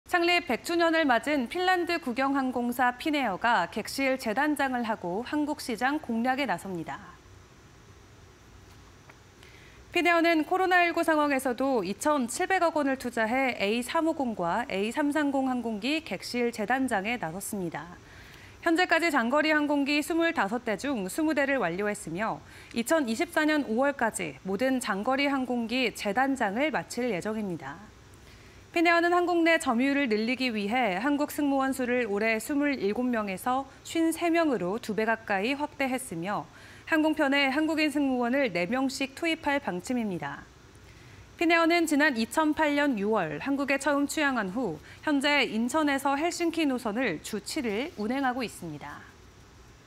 창립 '100주년' 핀에어... 韓 승무원 늘리고 맞춤 서비스 강화 (20231114)
# 창립 '100주년' 핀에어... 韓 승무원 늘리고 맞춤 서비스 강화